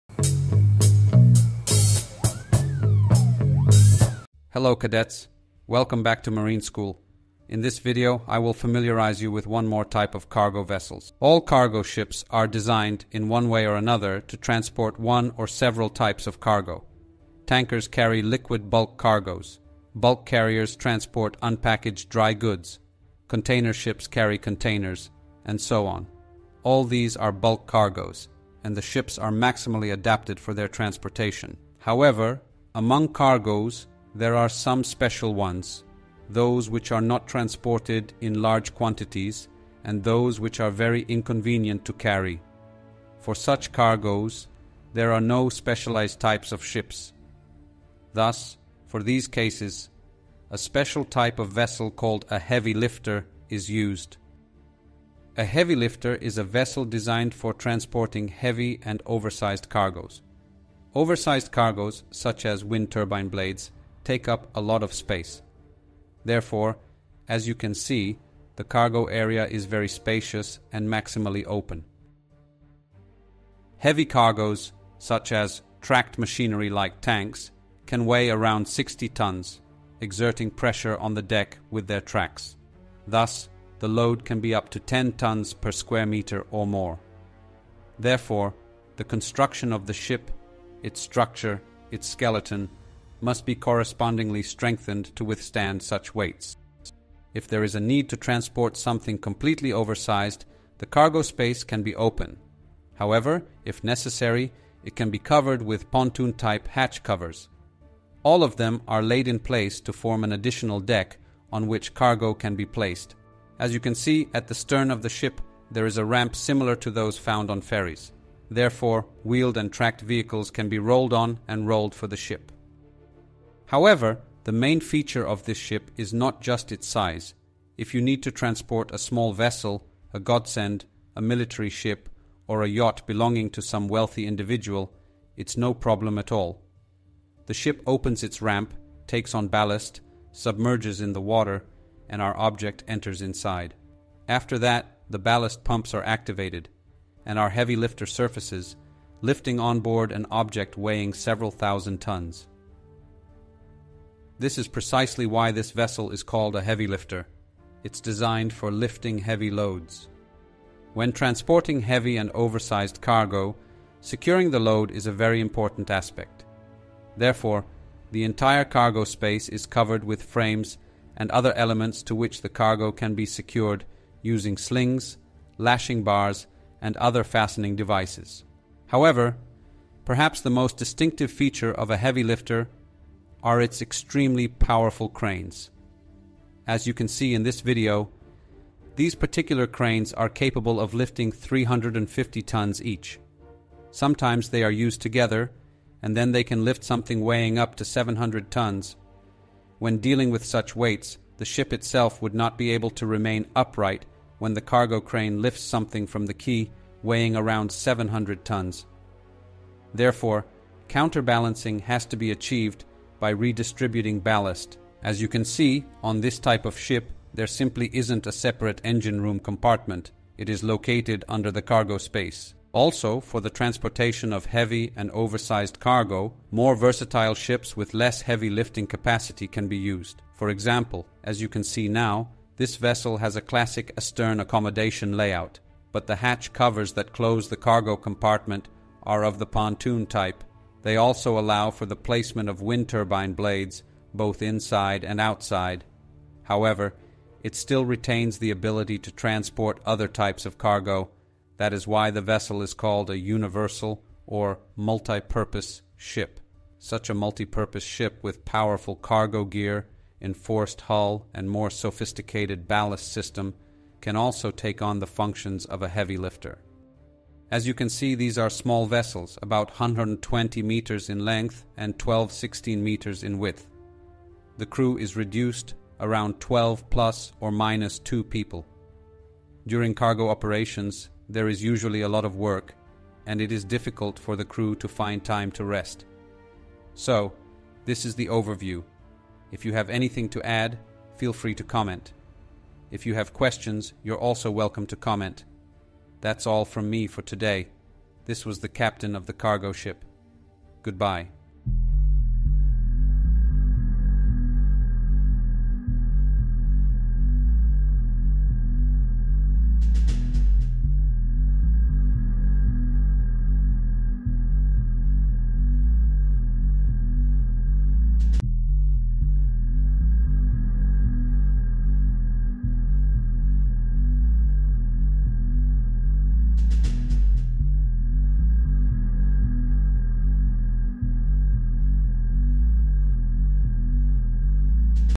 Heavy lifter: complex oversized project cargo transporter.
Overview of design and purpose of multipurpose heavy lift ships. Hull, Gear, Structure, Machinery and cargo opearations.